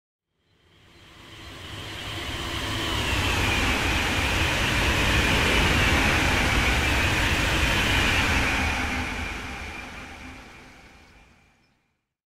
Eurostar e300 373015 & 373016 @ Aix en Provence
we see a Class 373 e300 speeding at Aix en Provence.